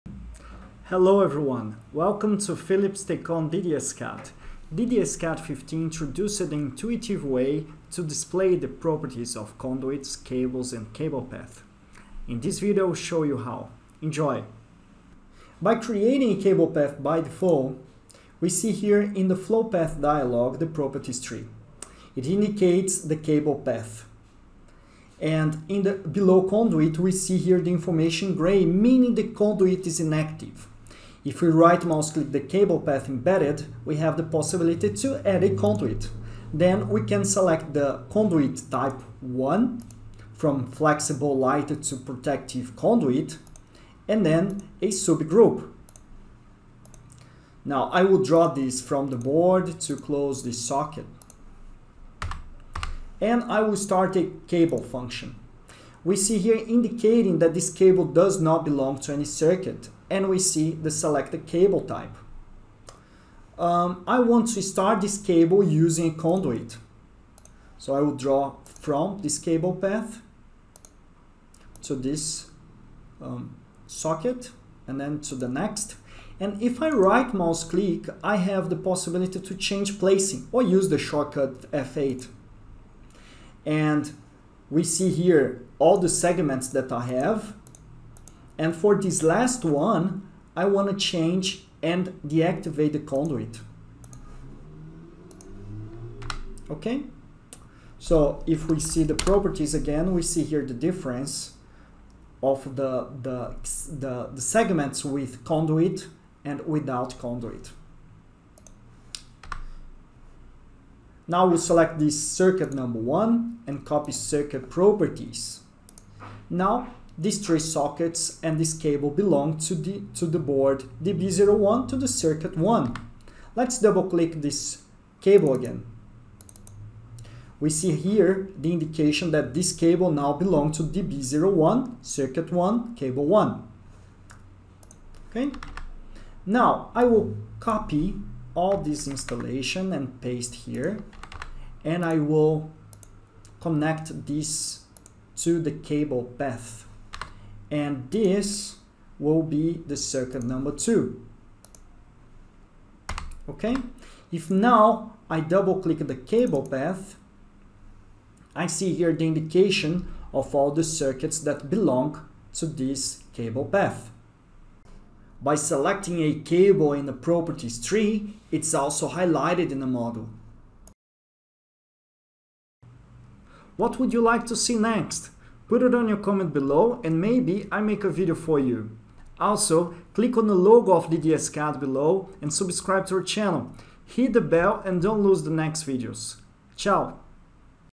Conduit and Cable New Properties Tree in DDScad
With the introduction of DDScad 15, a new dialog is also implemented. This not only includes the most important information about the cable path, cable or conduit, but also offers the possibility to implement changes directly. Moreover, it also offers a better overview of the various cable paths, cables and conduits.